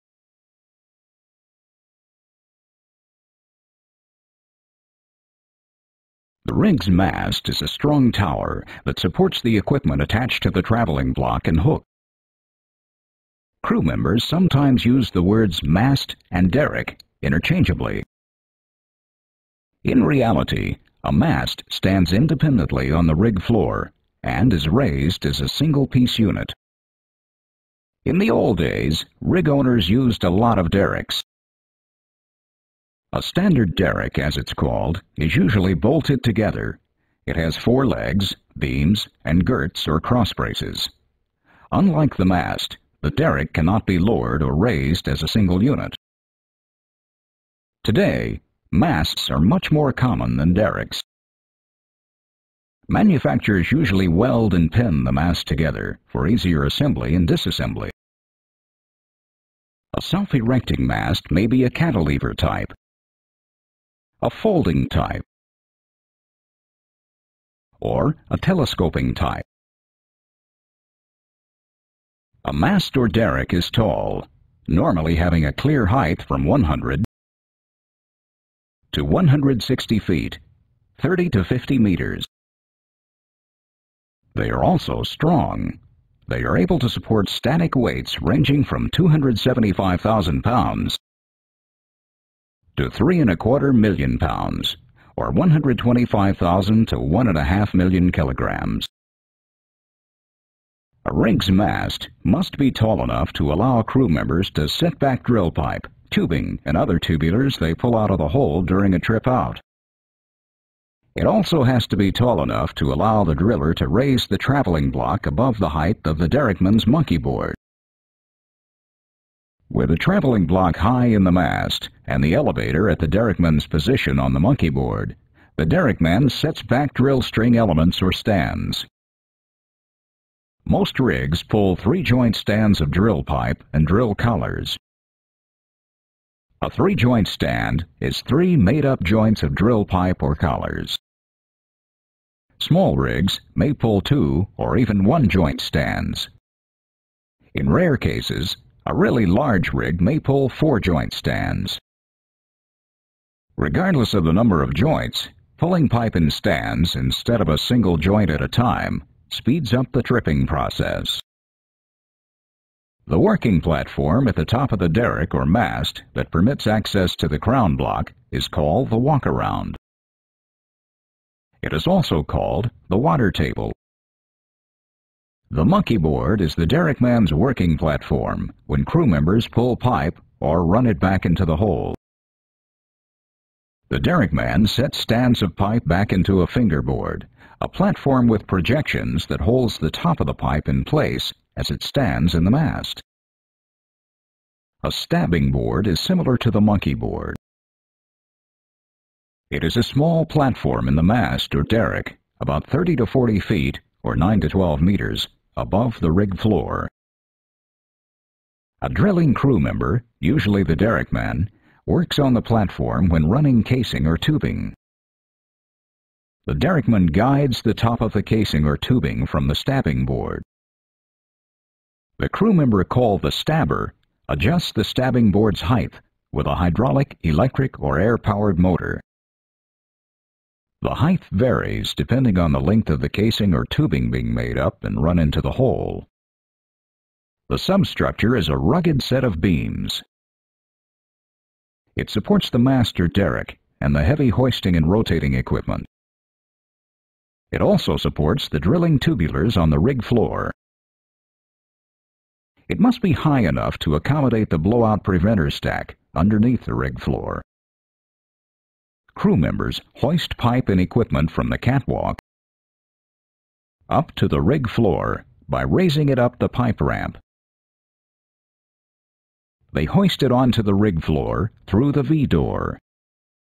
Rotating Equipment, Mast and Substructure. Part II
With this video you should be able to:
* Describe the mast and the substructure and explain their purpose.